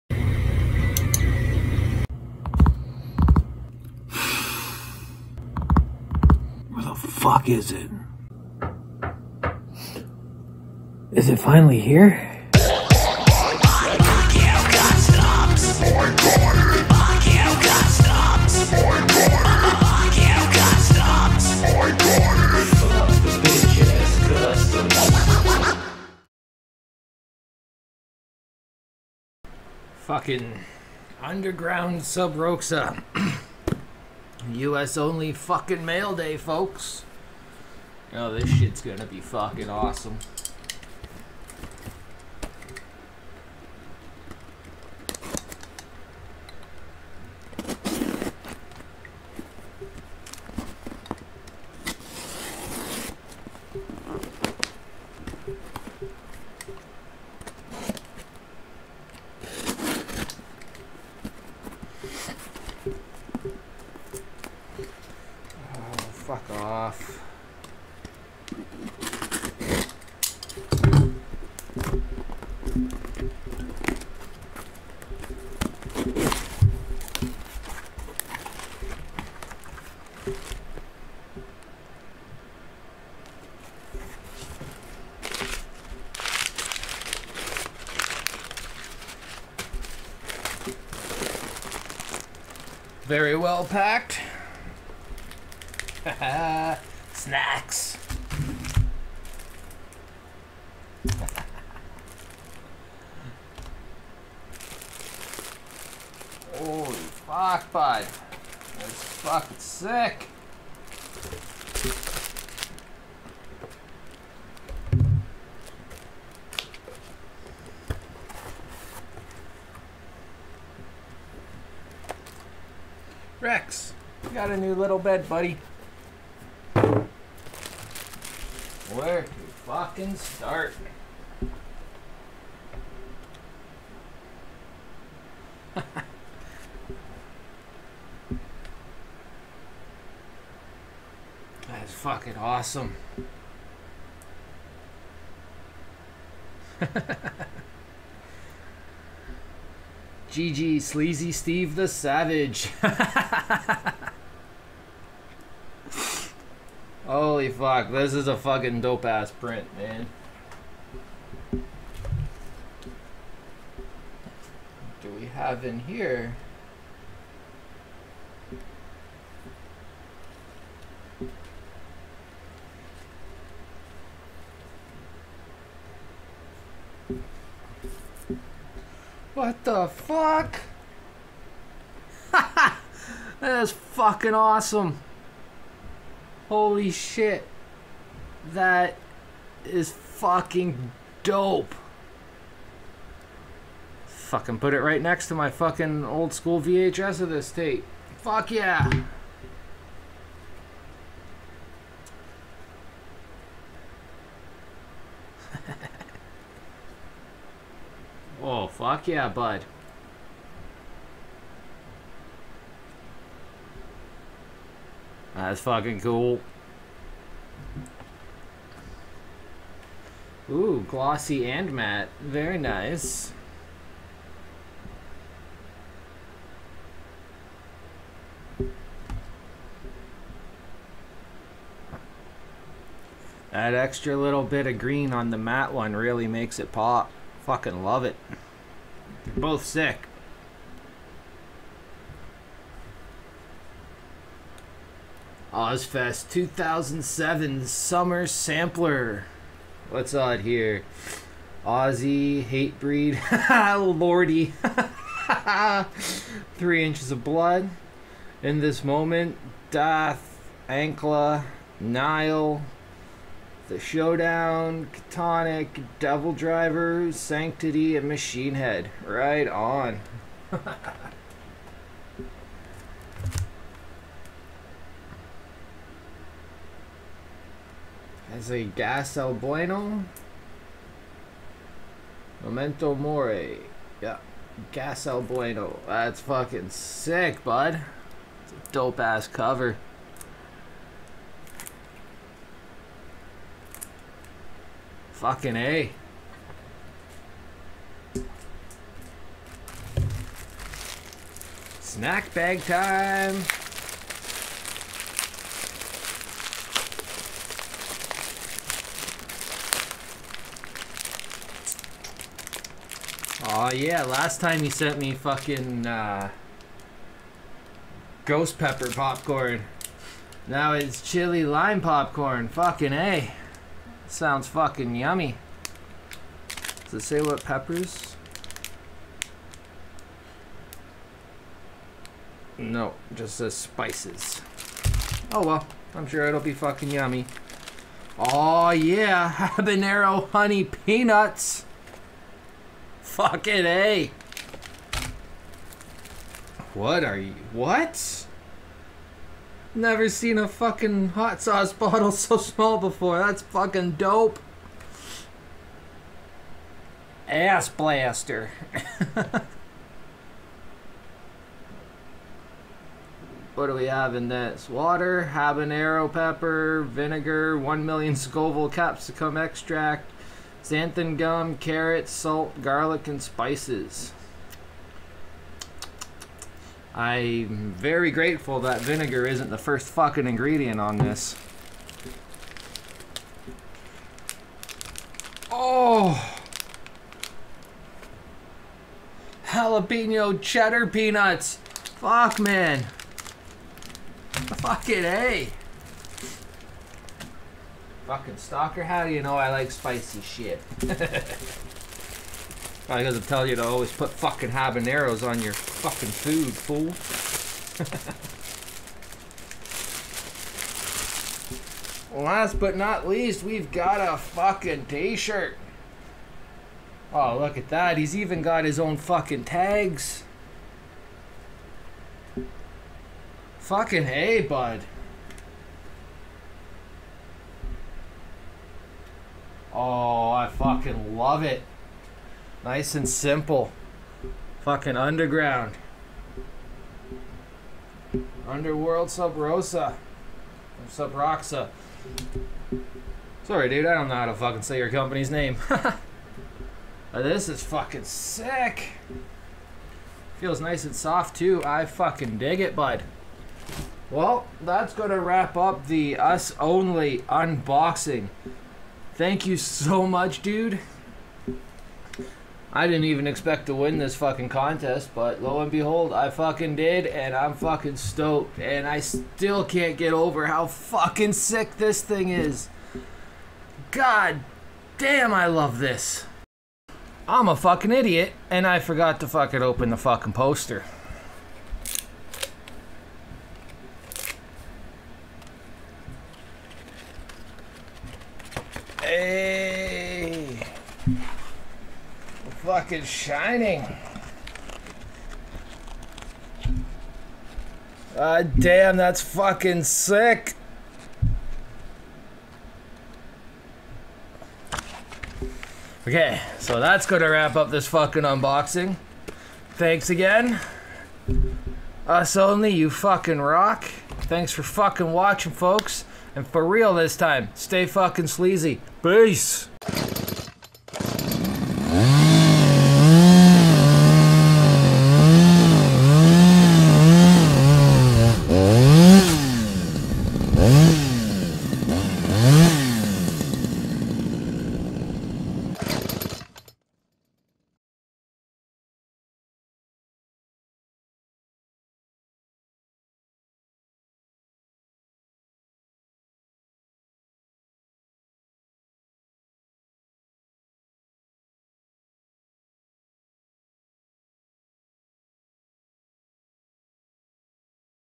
SHC: Unboxing - Prize Package from UxSxOnly and his Underworld Subroxxa shop
Won a draw over on Instagram last week and here's the unboxing of the prizes won from the homie UxSxOnly and the Underworld Subroxxa shop.

Upcoming project can be checked out here...

Intro Edit and Music by ZS